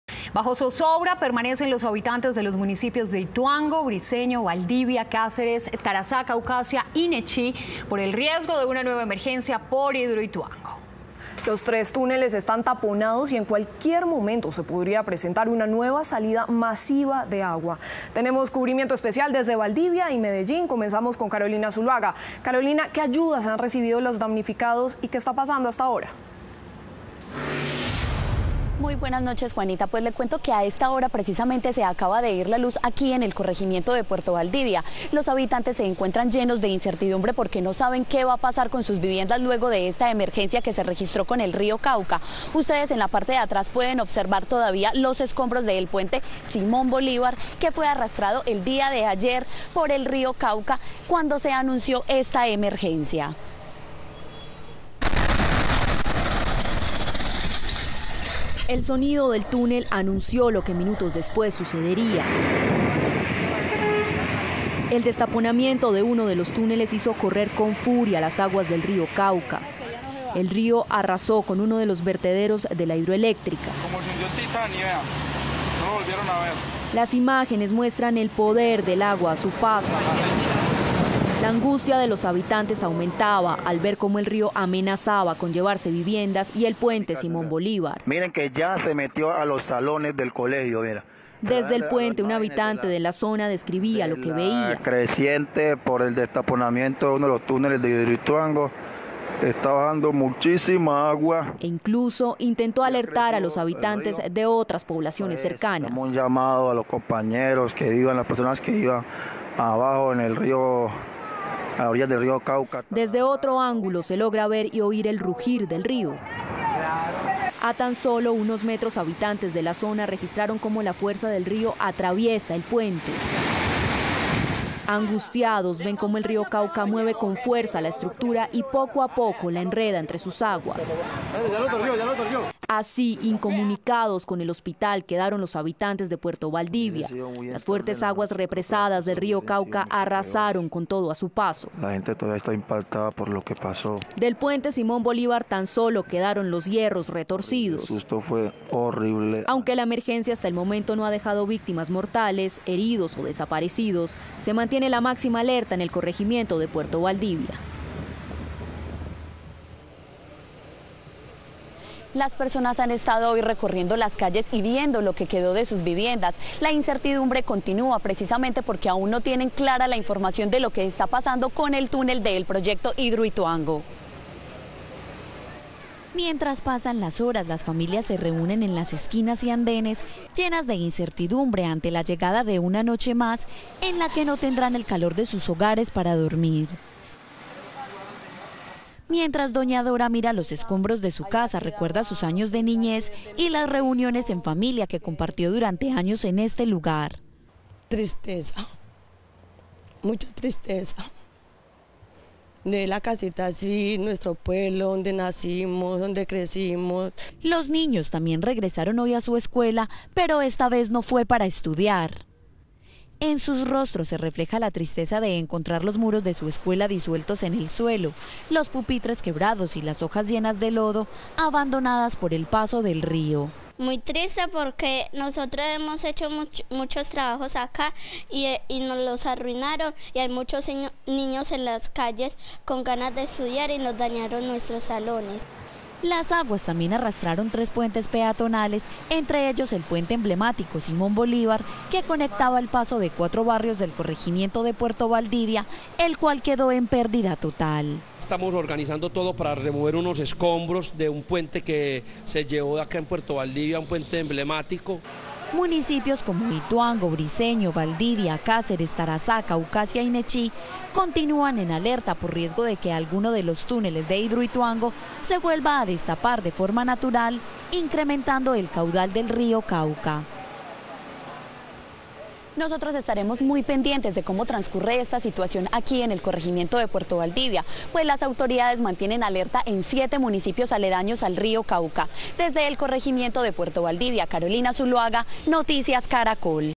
Zozobra en el Bajo Cauca antioqueño por posible nueva crecida del Cauca| Noticias Caracol
Mientras pasan las horas las familias comienzan a reunirse llenas de incertidumbre por la llegada de una noche más en Puerto Valdivia.

Mientras doña Dora Taborda mira los escombros de su casa recuerda su niñez y las reuniones en familia que compartió durante años en este hogar.